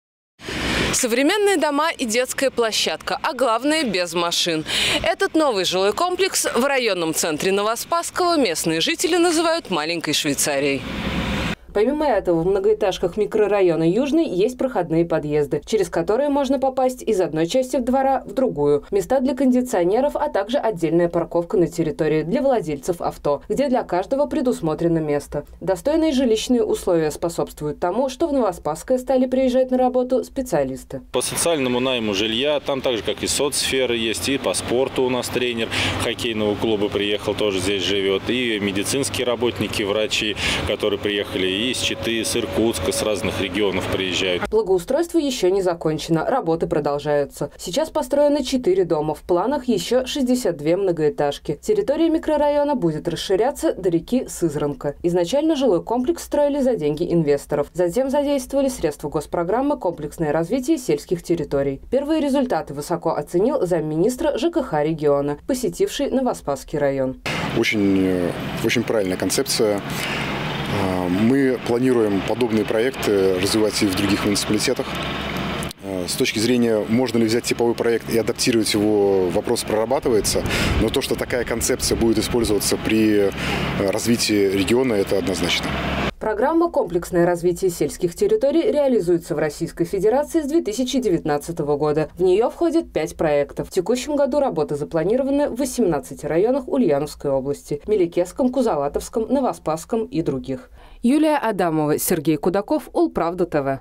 Швейцария в Новоспасском. В ульяновских селах строят современное жилье
В Новоспасском Ульяновской области продолжают возводить новый микрорайон. Благоустроенная территория привлекает не только местных жителей, но и ценных профессионалов для рабочего посёлка. Новейшие решения воплотили в жизнь и в самих новостройках, и во дворах.